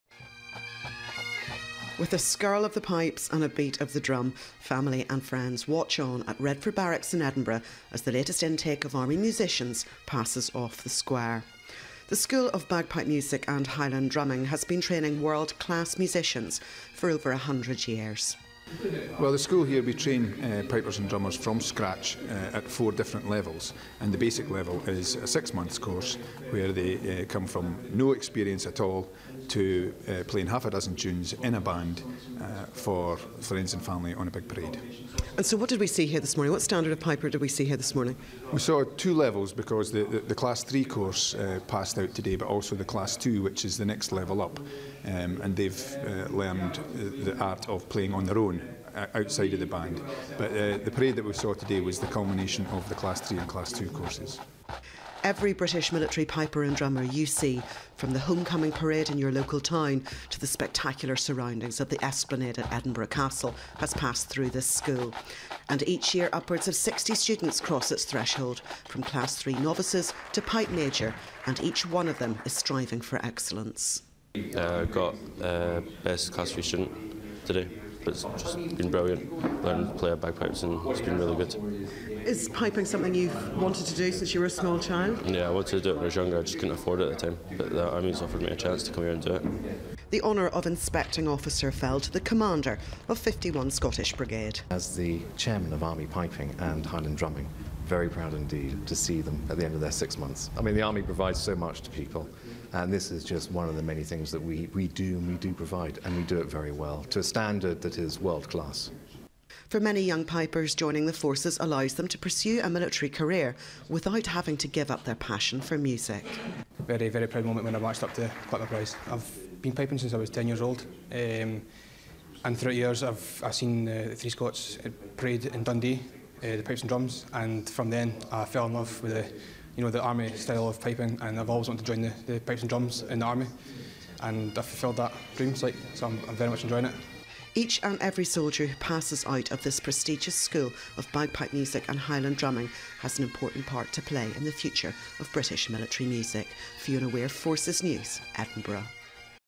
School Of Bagpipes And Highland Drumming | Forces TV
The School of Bagpipe Music and Highland Drumming has been training world-class musicians for over a hundred years. 31 Soldiers from Scottish, Irish and Ghurka Regiments have spent the last six months being tutored in the art of piping and drumming by experts at the school at Redford Barracks in Edinburgh.